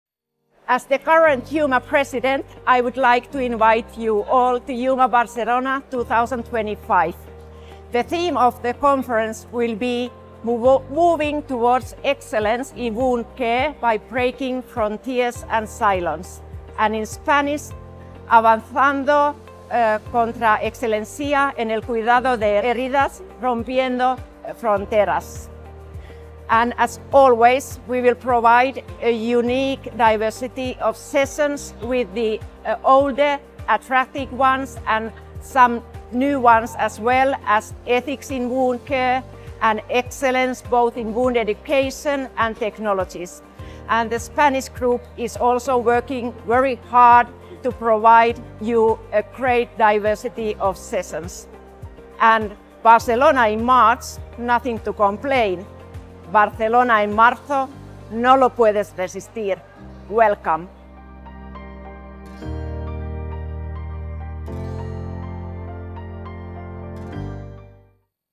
The theme of EWMA-GNEAUPP 2025 Conference
The theme of the EWMA-GNEAUPP 2025 Conference is “Moving towards excellence in wound management by breaking frontiers and silos”.

The conference will feature high quality sessions & presentations, numerous workshops and e-poster presentations as well as a large industry exhibition where you can meet various companies within wound care.

We hope to see you in Barcelona! ✨